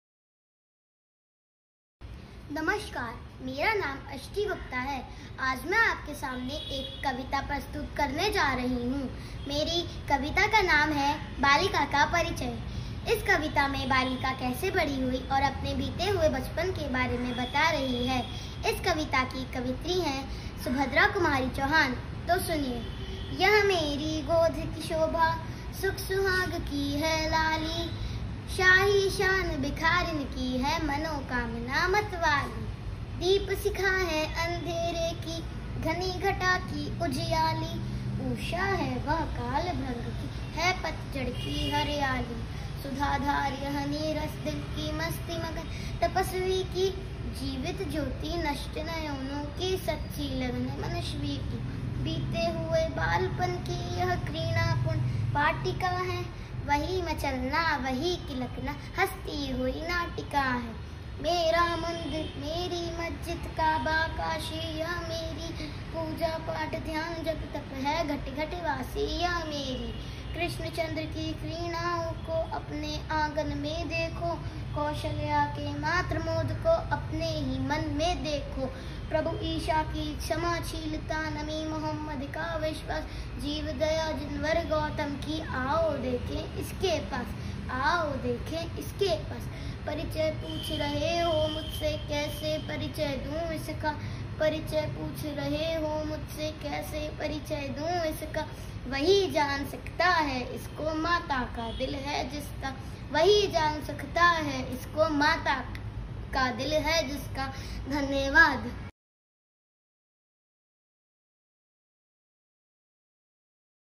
Kavigram - Kachpan || Ashti Gupta || बालिका का परिचय || Subhadra Kumari Chauhan
'कचपन' बचपन को कविता के संस्कार देने के उद्देश्य से कविग्राम द्वारा शुरू की गई एक श्रृंखला है।   'कचपन' एक प्रयास है तोतली बोलियों में साहित्य की गौरवशाली परम्परा के यशगान को कलरवित करने का। 'कचपन' एक कोशिश है; क-से कबूतर और K- for kite वाली पीढ़ी को 'कविता' जैसे शब्द तक पहुँचाने की।

कविग्राम को इस पुनीत प्रयास के यज्ञ में आप सब की ढेर सारी प्रविष्टियाँ पावन समिधाओं के रूप में प्राप्त हुईं हैं। आइए; इस पुनीत यज्ञ की पावन आहुति के रूप में सुनते हैं 8 वर्षीय अष्टि गुप्ता  मुख से सुभद्रा कुमारी चौहान जी की कविता - 'बालिका का परिचय'।

आप इस वीडियो को जितना अधिक लाइक तथा प्रसारित करेंगे उतना ही इन बच्चों को कविताओं से जुड़ने का प्रोत्साहन मिलेगा।